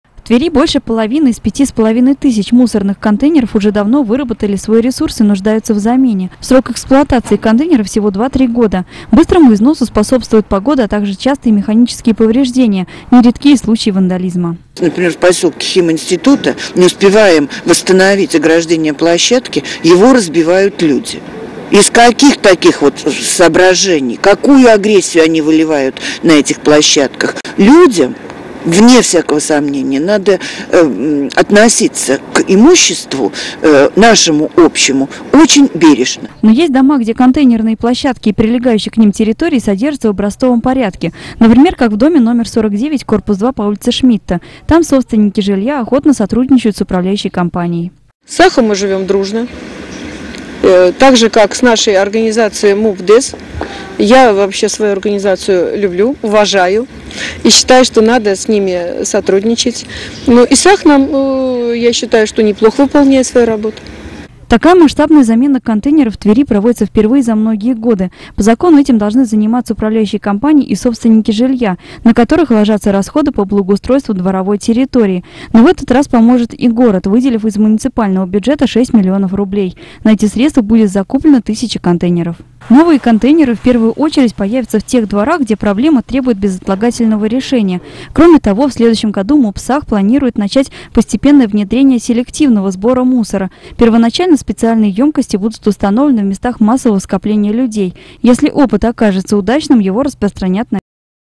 Тысяча контейнеров для Твери
Тысяча новых мусорных контейнеров появятся в Твери уже в ближайшее время. Они станут первыми, но не единственными. На масштабную замену из городского бюджета выделено 6 миллионов рублей. Новые контейнеры, в первую очередь, будут распределяться по жилому фонду, находящемуся в ведении 6 крупнейших управляющих компаний.